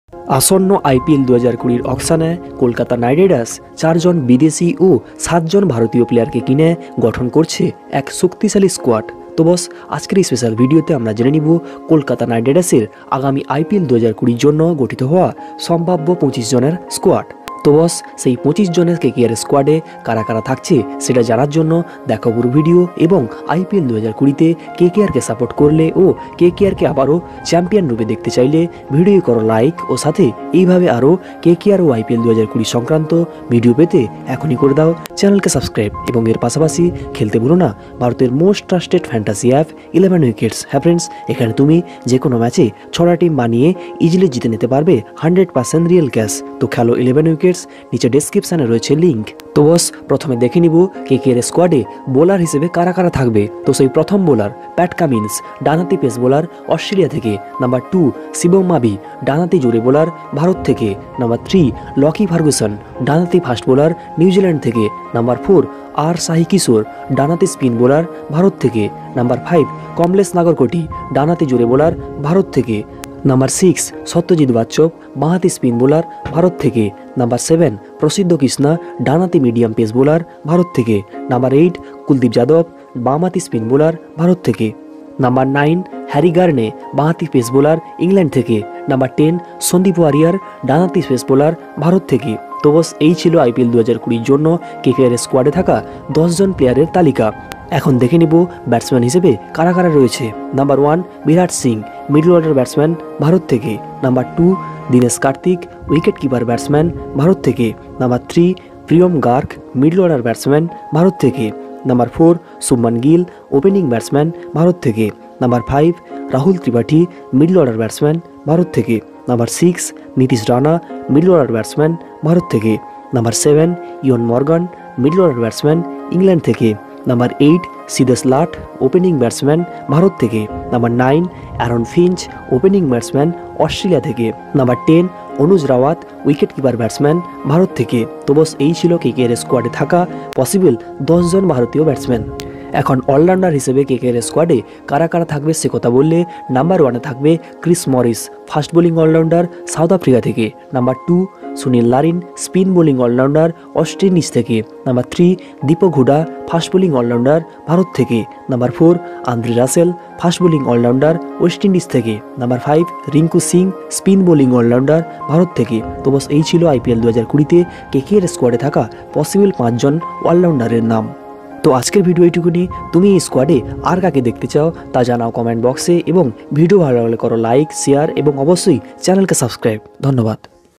IPL 2020 : কোলকাতা নাইট রাইডার্সের 25 জন প্লেয়ারের ফুল স্কোয়াড || দেখুন সম্ভাব্য দলে কারা থাকছে
In this video I am telling about upcoming indian cricket tournament ipl2020 and ipl auction 2020 . where I have discussed about one of the most successful iplteam kolkata knight riders and their ipl 2020 squad.

Also , in this video we are discussed kolkata knight riders team squad 2020 and all kkr player list 2020.